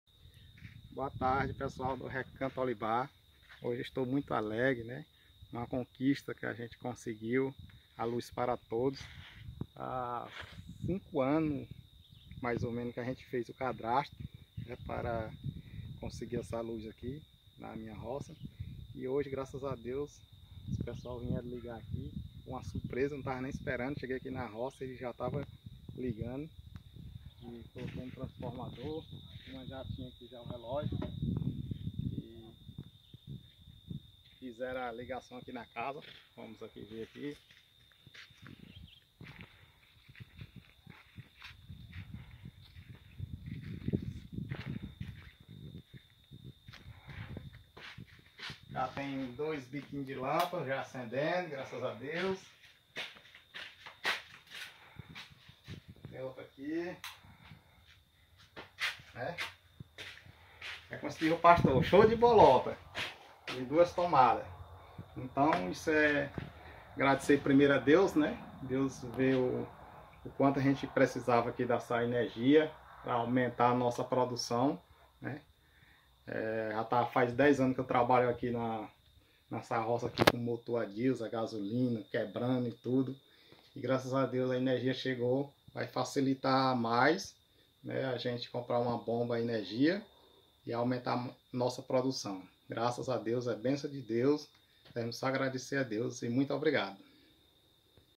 Energia elétrica na roça
Ola gente boa, espero em Cristo Jesus que você e sua família estejam em paz e com plena saúde.
Veja a alegria desse agricultor ao receber energia elétrica na rua roça.